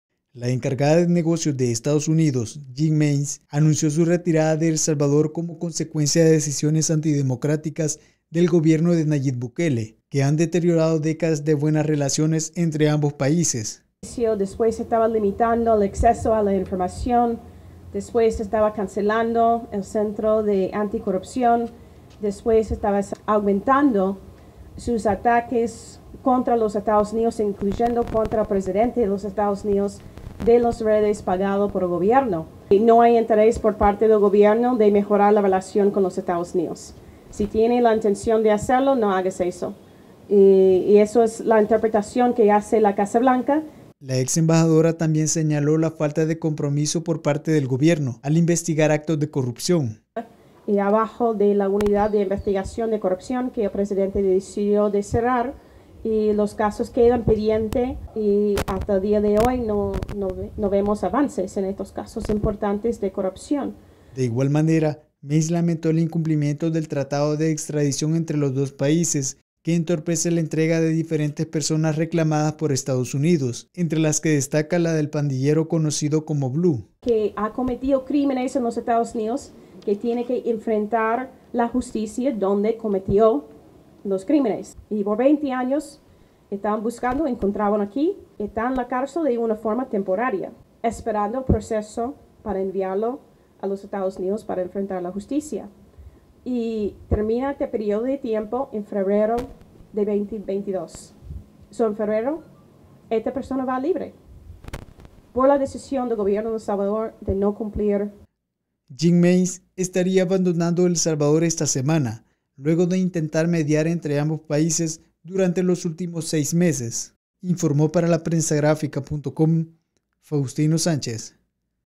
Gobierno de Nayib Bukele no tiene intenciones de mejorar las relaciones con EUA: Jean Manes
La encargada de Negocios de Estados Unidos, Jean Manes, anunció su retirada de El Salvador como consecuencia de decisiones antidemocráticas del Gobierno de Nayib Bukele, que han deteriorado décadas de buenas relaciones entre ambos países.

¡Visítenos!